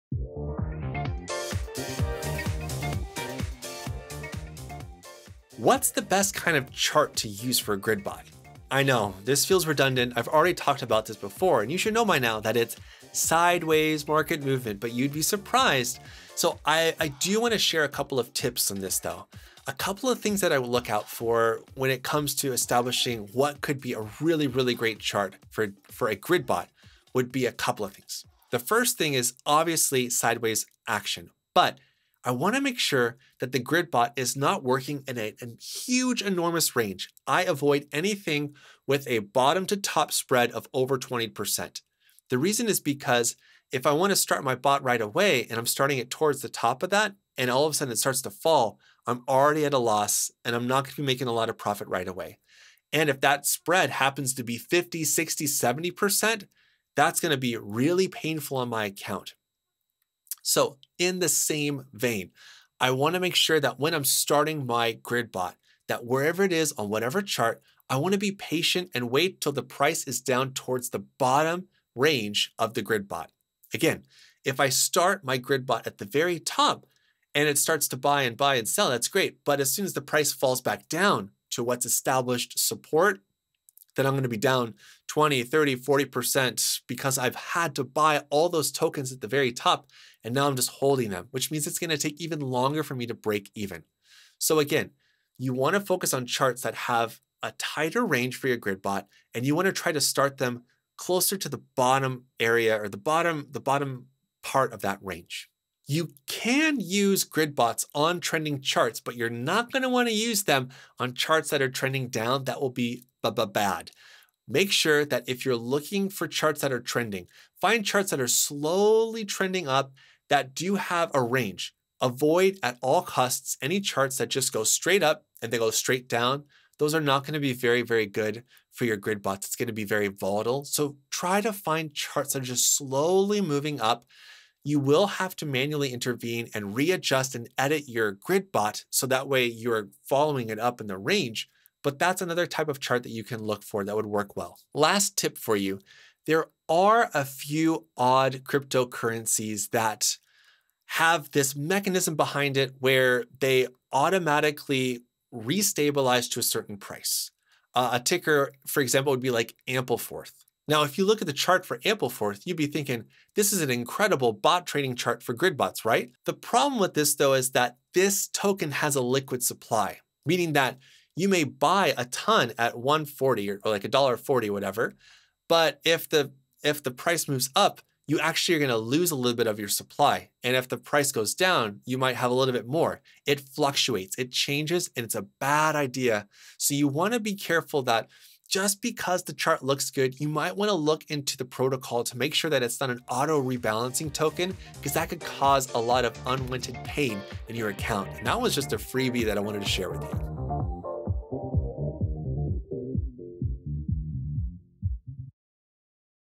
3Commas Academy Grid Bot Part 3
Discover 3Commas Grid Bot, the perfect tool for trading in sideways markets. MooninPapa from The Better Traders shows you how to configure and optimize your Grid Bot so you can get started fast and trade confidently.